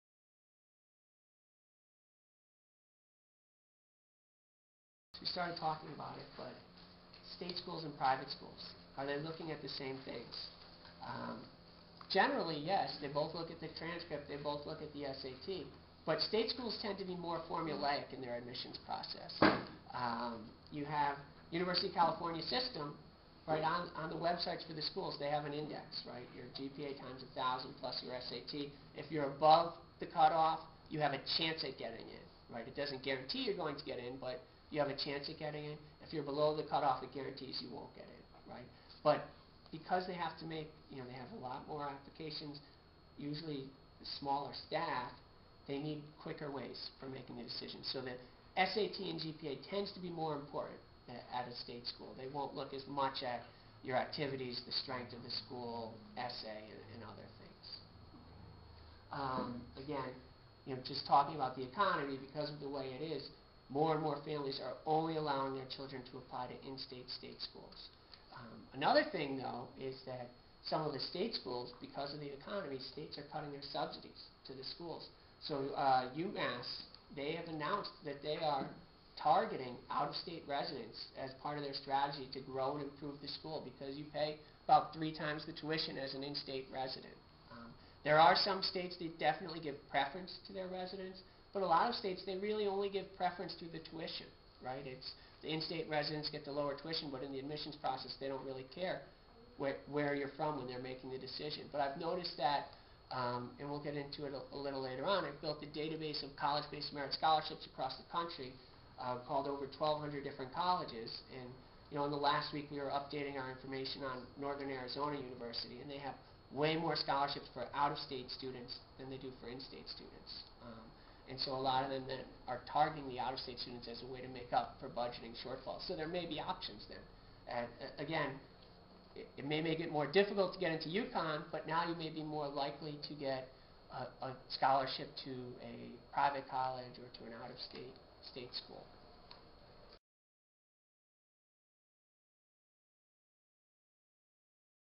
9 Things You Need to Know About College Planning: Lesson 5
In this video, James Maroney of discusses between how state schools and private colleges review students' applications.